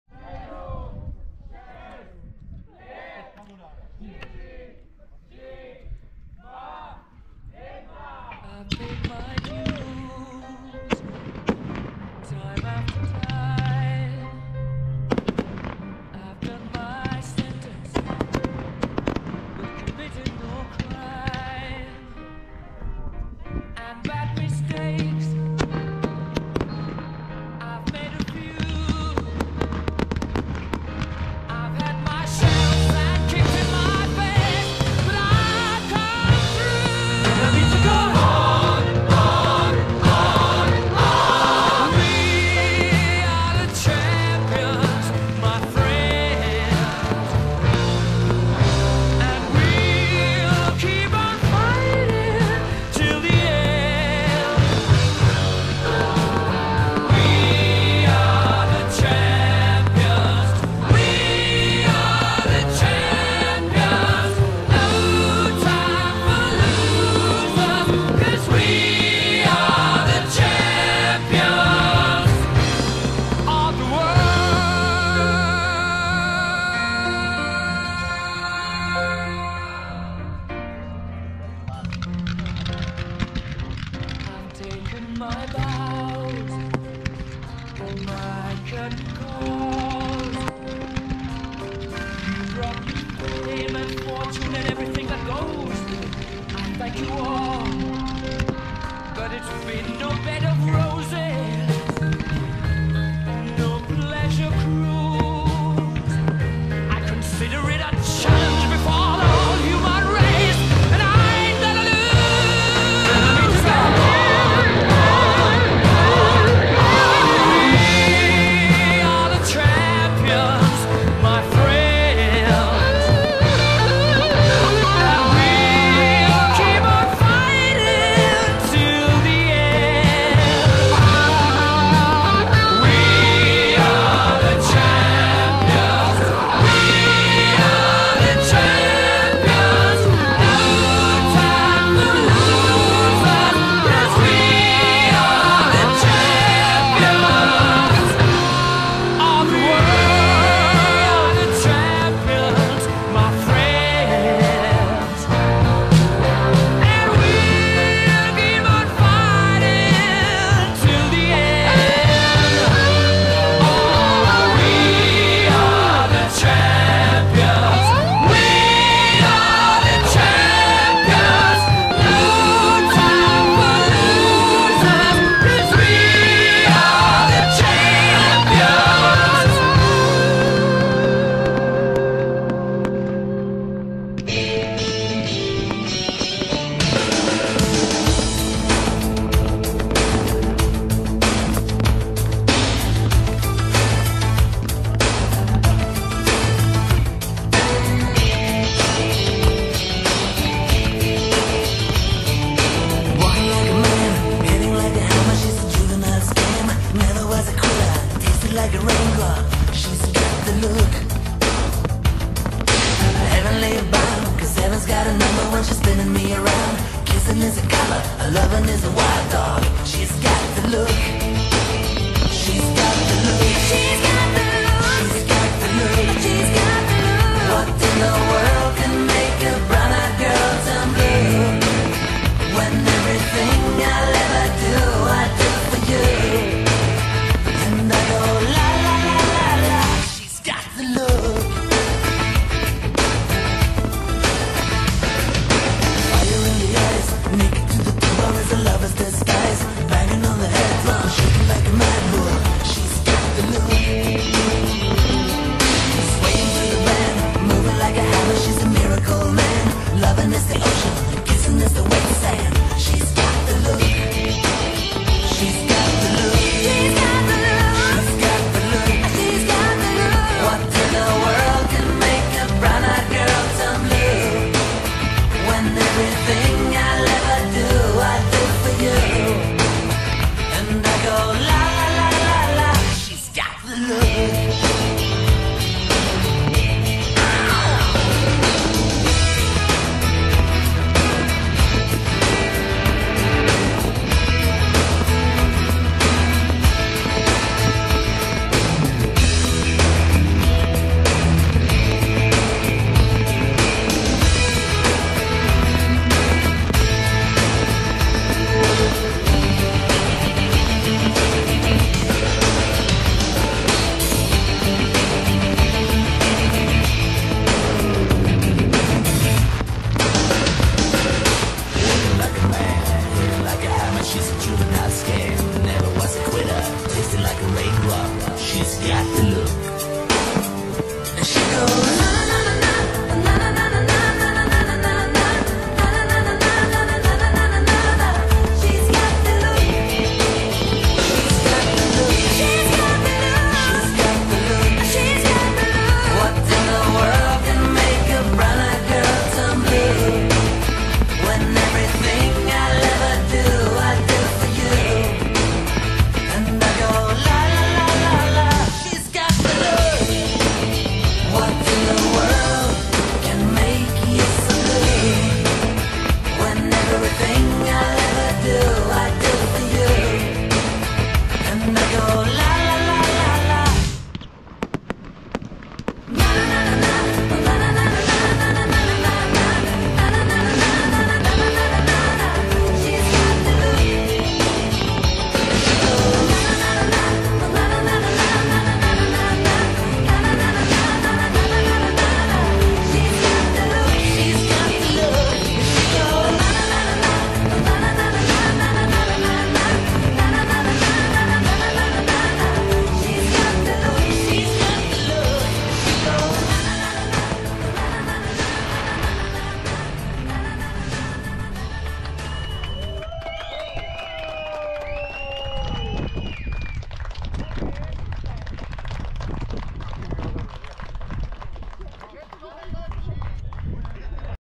Ohňostroj Horní Benešov 2023 - Umělecká Agentura LEV
Narozeninový ohňostroj hudbou jen podbarvený, pro soukromou oslavu privátní osoby, odpálený na fotbalovém hřišti v Horním Benešově.
HQ/UHD 4K Go Pro Hero Black 10